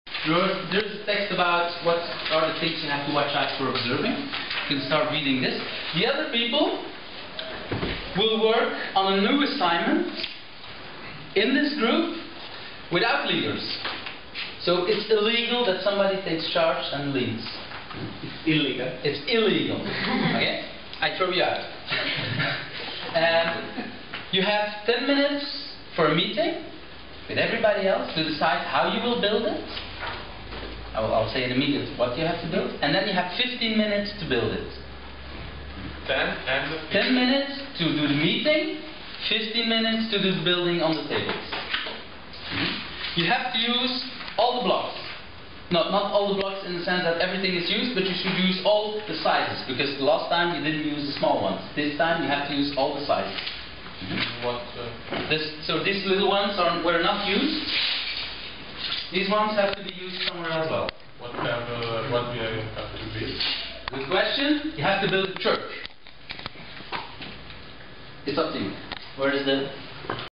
The leadership game @ essap 2008
people will experiment with how they behave as leaders and how they interact with leaders with different leadership styles.